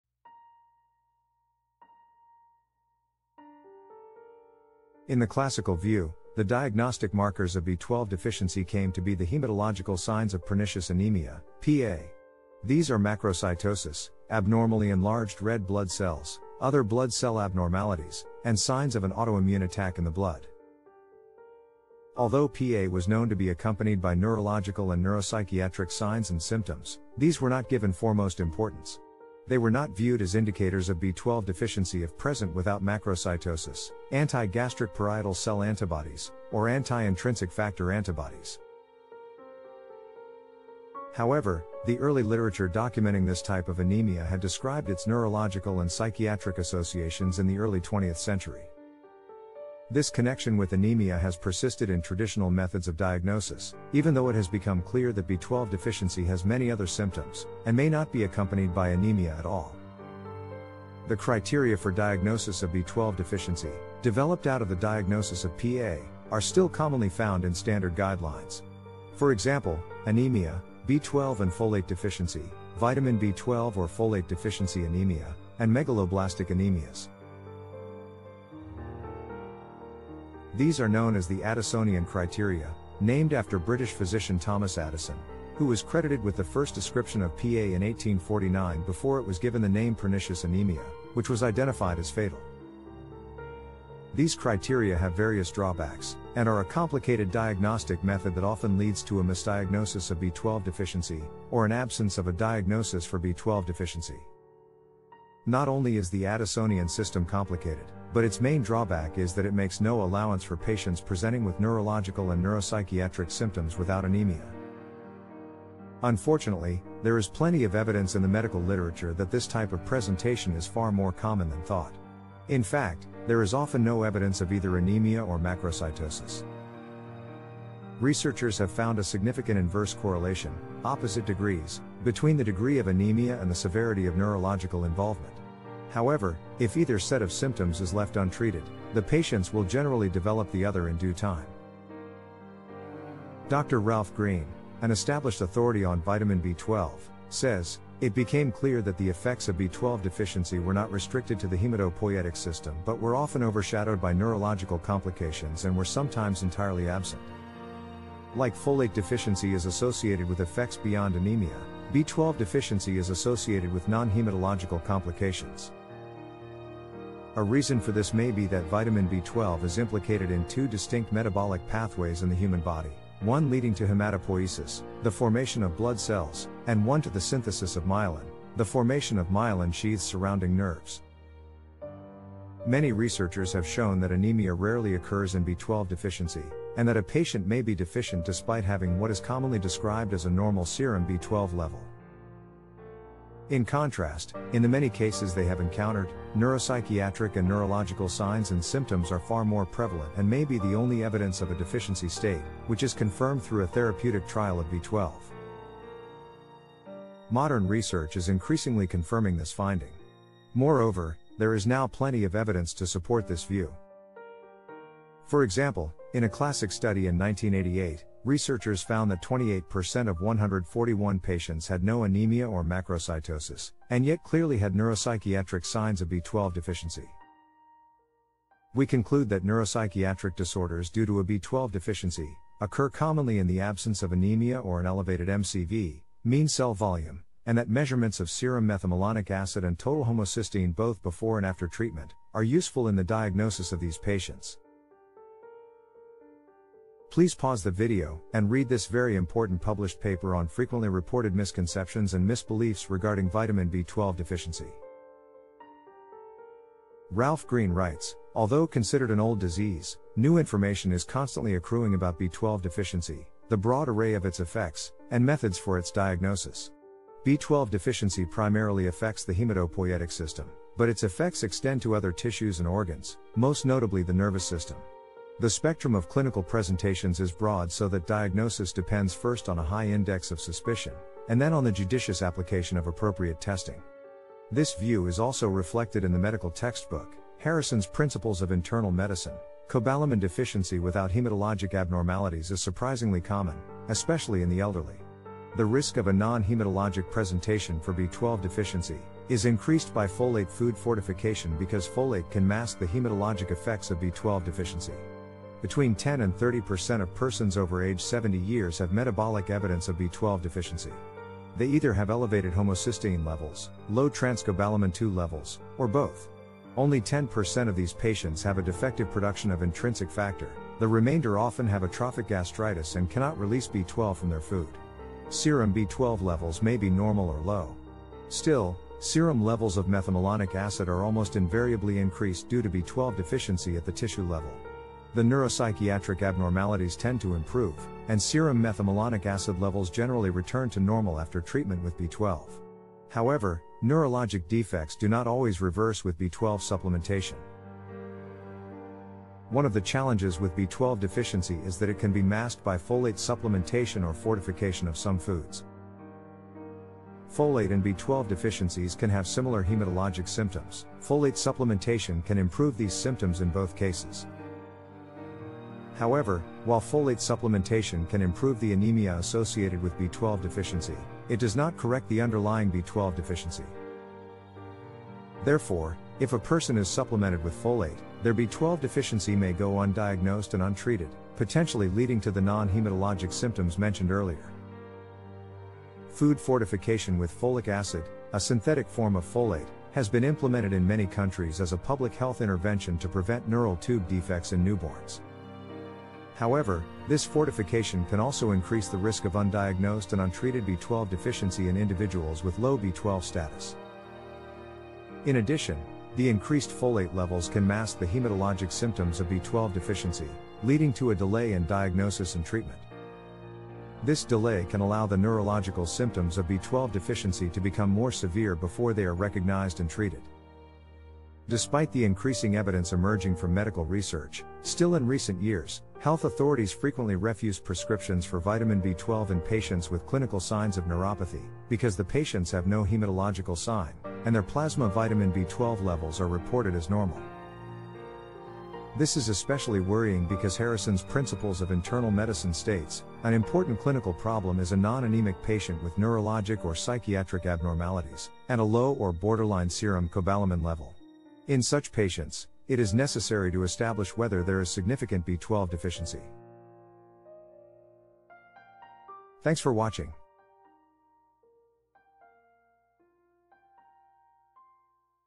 The Limitations of Addisonian Criteria: Diagnosing B12 Deficiency
Introducing a video series on the many problems with conventional B12 testing.

Part 1 explores the traditional view of diagnosing B12 deficiency through the Addisonian criteria, focusing on hematological signs of pernicious anemia. However, this approach often leads to misdiagnosis or missed diagnosis of B12 deficiency, as the neurological and neuropsychiatric symptoms that often accompany B12 deficiency may not be present in conjunction with anemia.

This video examines the limitations of the Addisonian criteria and provides insight into how modern research increasingly recognizes the diverse symptoms associated with B12 deficiency. Additionally, this segment discusses the impact of folate food fortification and how it masks B12 deficiency, leading to a misdiagnosis or no diagnosis of a B12 deficiency. Join us for Part 1 of this informative series on B12 deficiency.